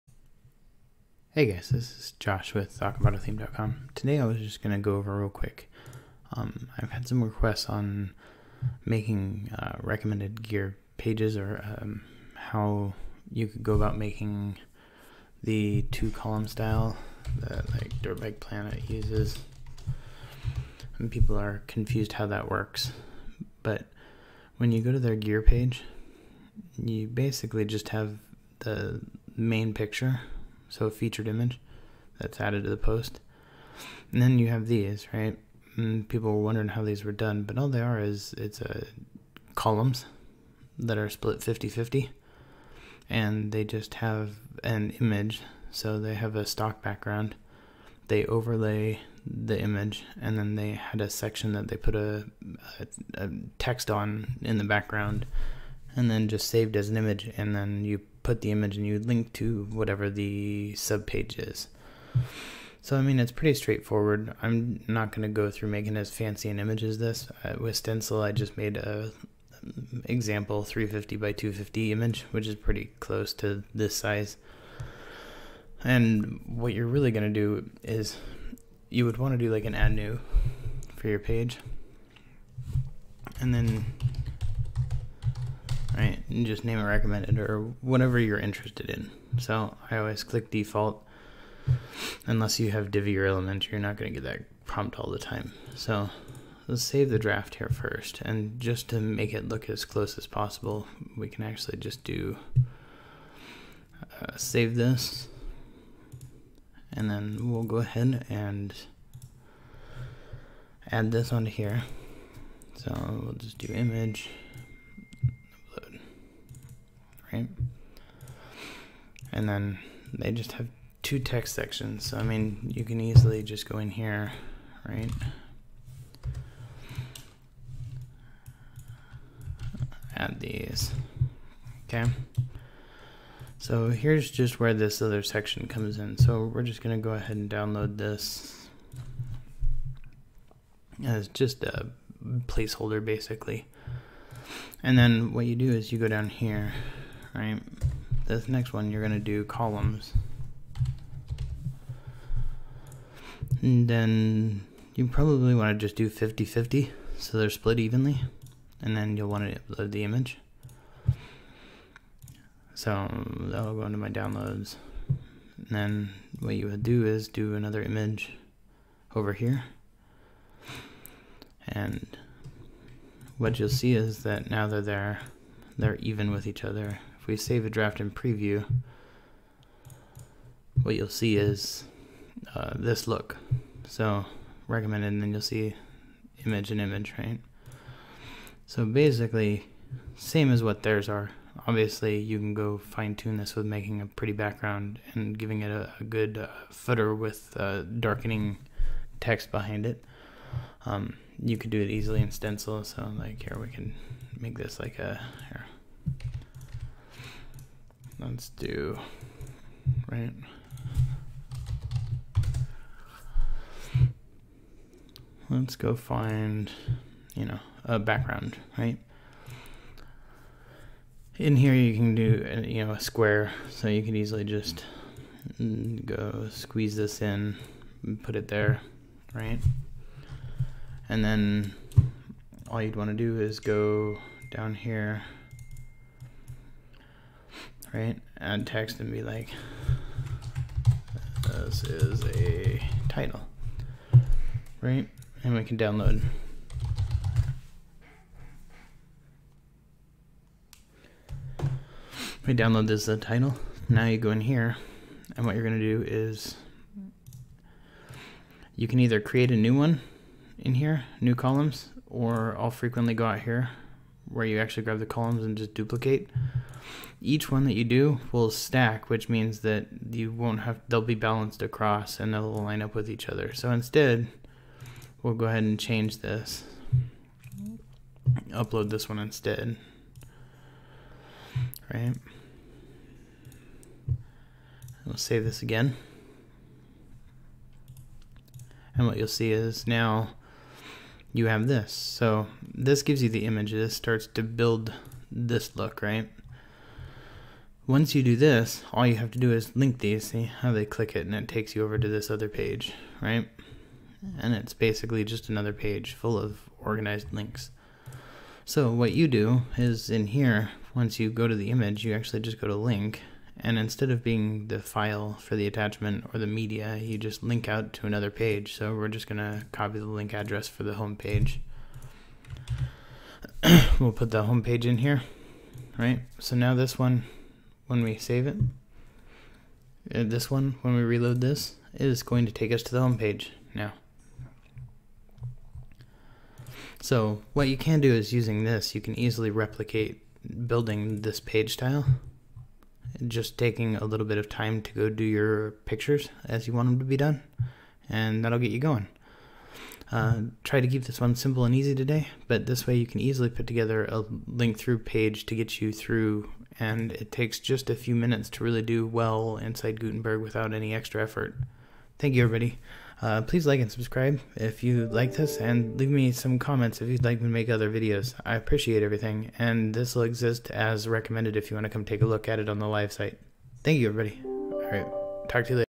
Build Acabado Recommended Product Page in Gutenberg
Just wanted to note that I am compensated for referring traffic and business to Income School if you sign up, please message me and I can help you get setup fast!

In the video I use Stencil which I do recommended for the easy ability to use: